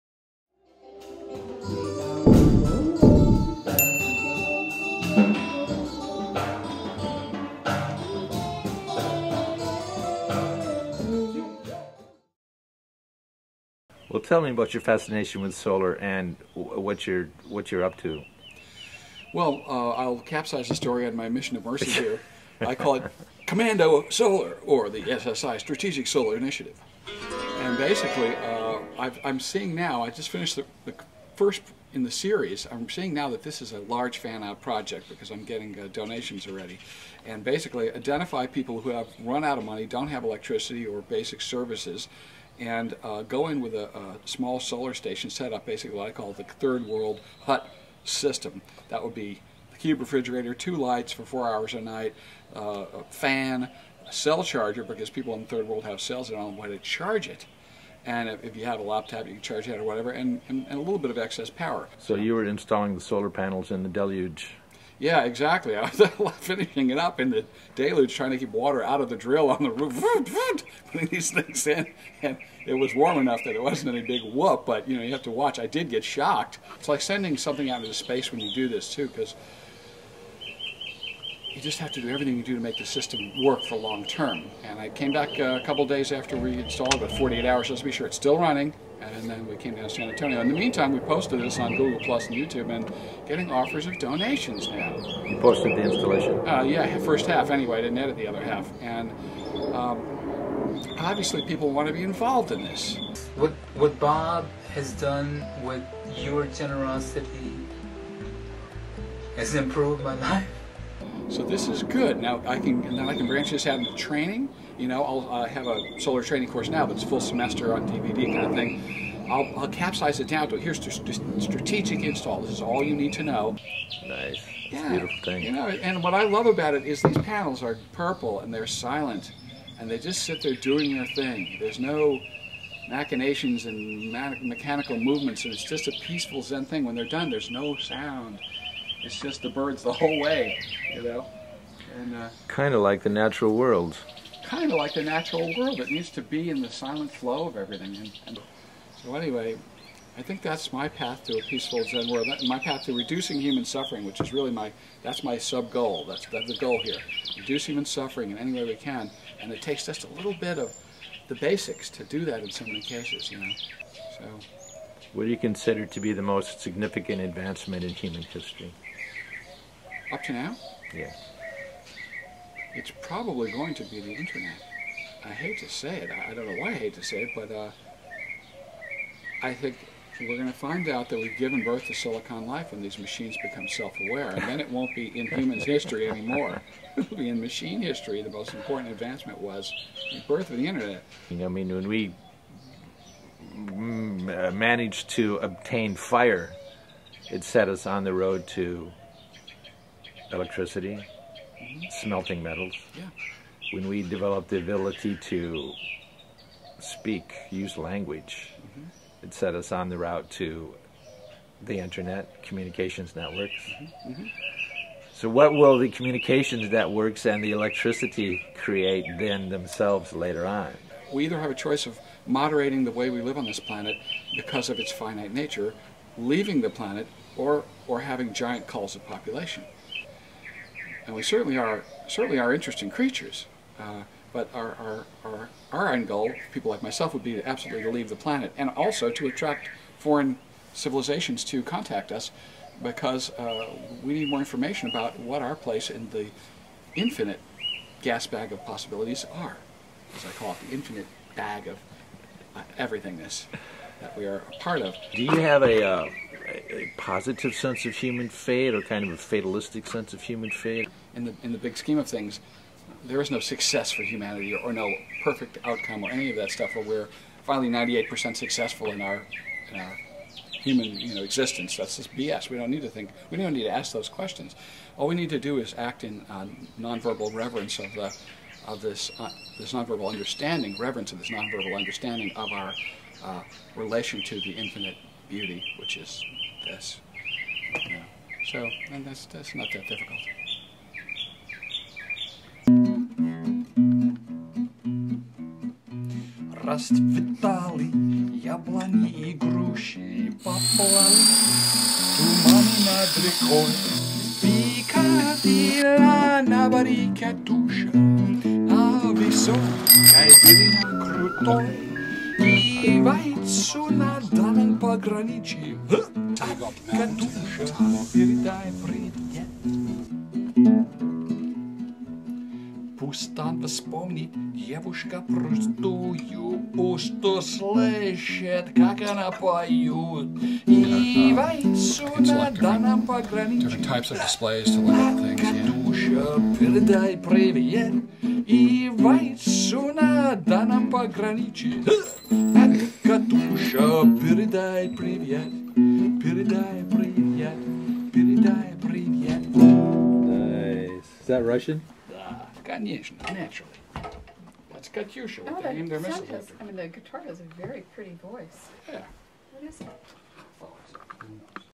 bob nagy solar commando
My friend and mentor Bob Nagy is really into solar.  He produces solar instructional DVDs, installs solar panels on houses, and an electric car powered by the sun and helps others who live off the grid to get power.